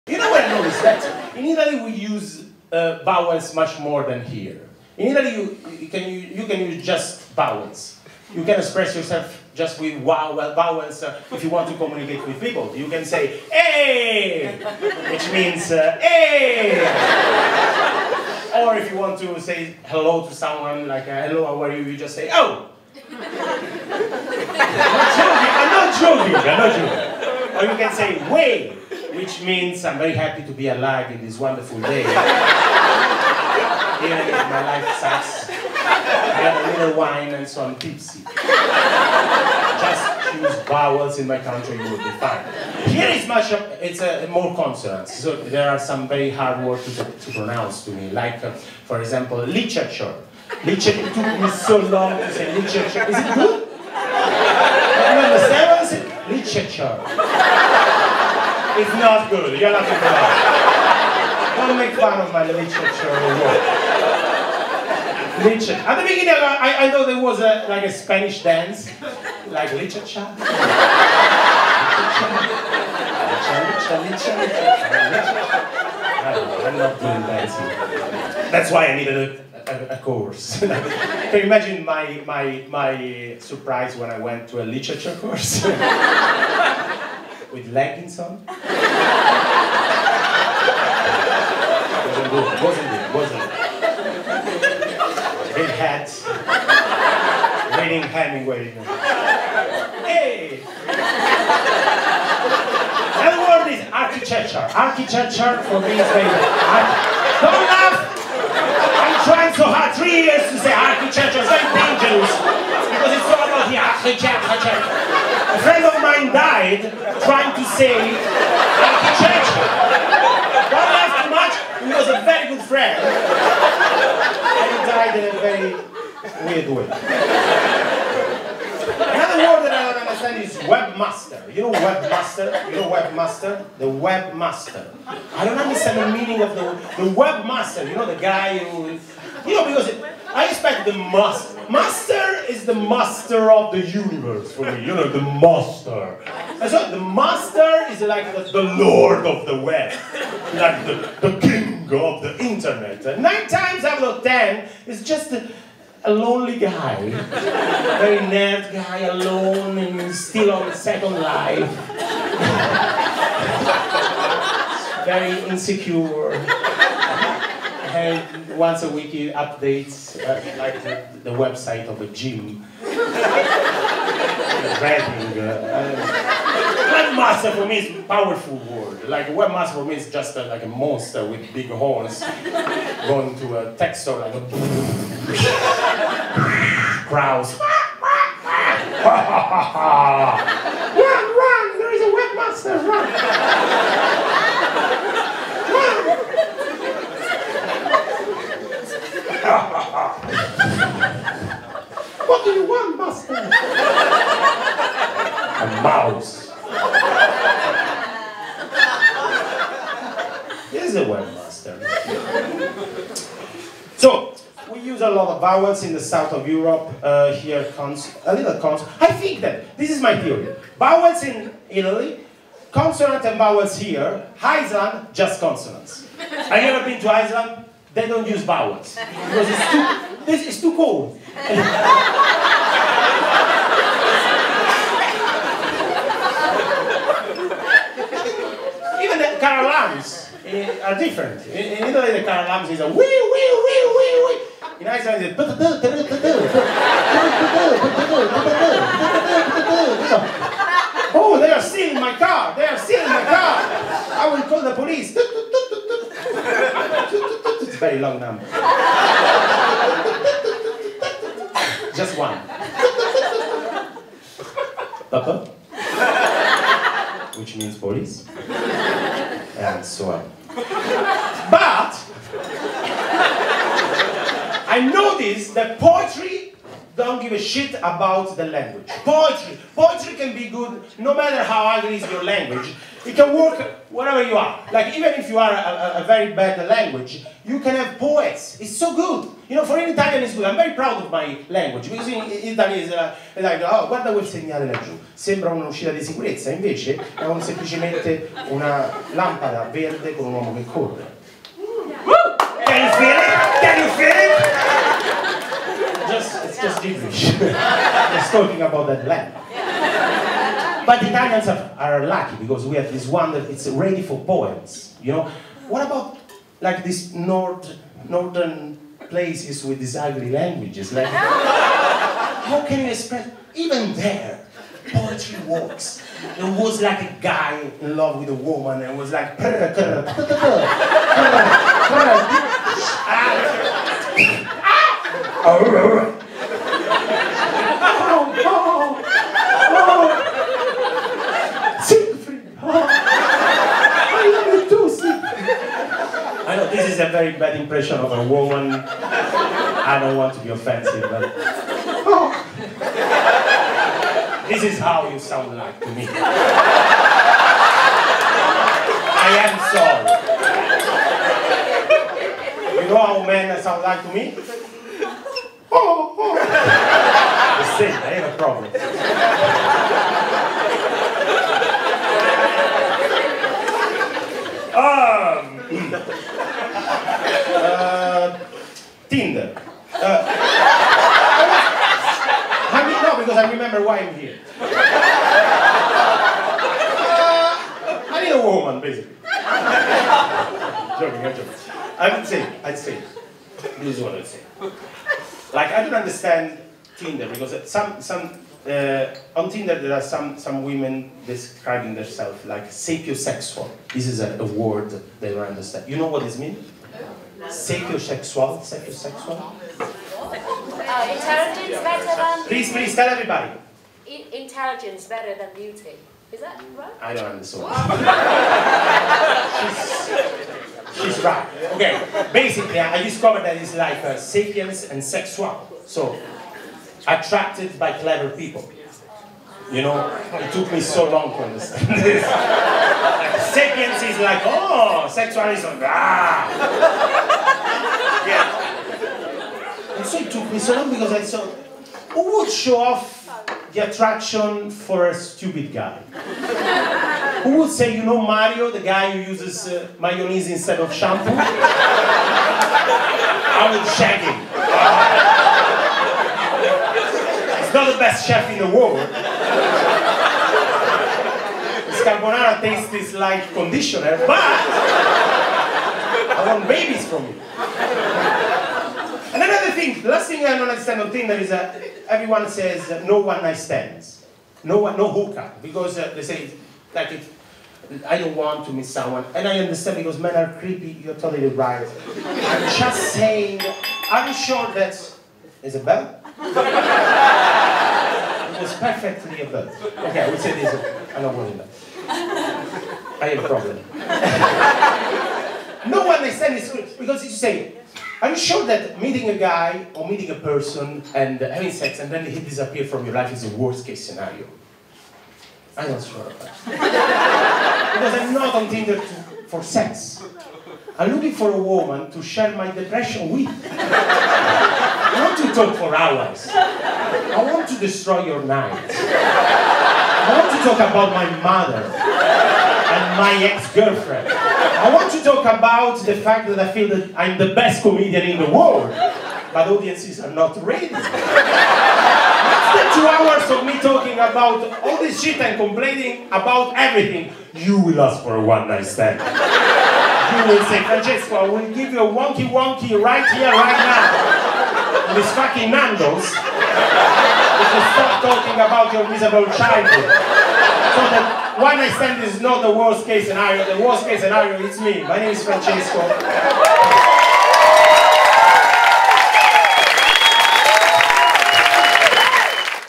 An Italian Comedian in London part 2 - Francesco De Carlo in English
Secondo video di una mia esibizione di stand up comedy a Londra.

Second video from a stand up set in London, Backyard Comedy Club (Bethnal Green)